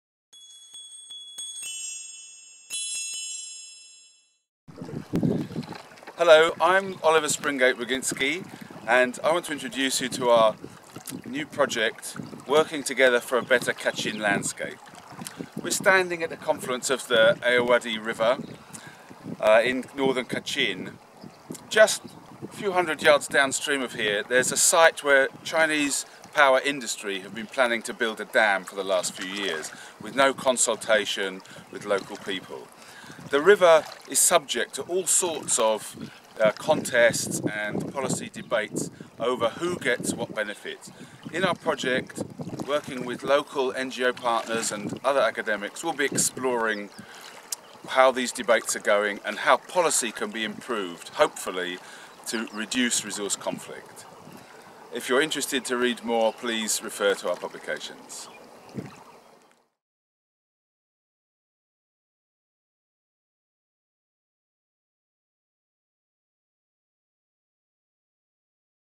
Meet WLE Greater Mekong's MK29 project
In the Upper Ayeyarwady Basin, Oliver Springate-Baginski of the Water Security Research Centre at the School of International Development at the University of East Anglia and a team of regional and global partners are working on "A Landscape Approach to the Upper Ayeyarwady River Basin: Building Inclusive Governance Processes to Address Resource Conflicts," coded MK29.